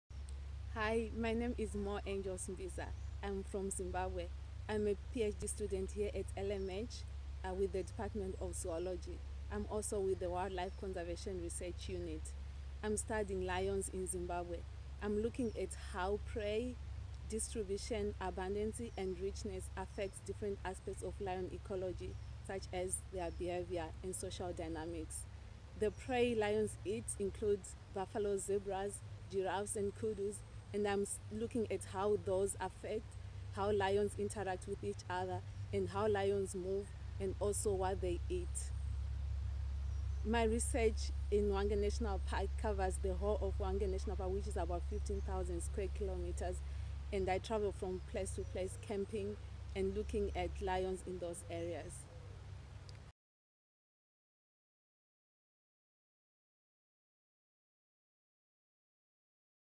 Moreangels' research - DPhil in Zoology, University of Oxford
Moreangels Mbizah, a DPhil graduate at Oxford, on her on-the-ground research on prey distribution in lion ecology. She looks at lion conservation in Zimbabwe, and is working with the WildCru Wildlife Conservation Research Unit.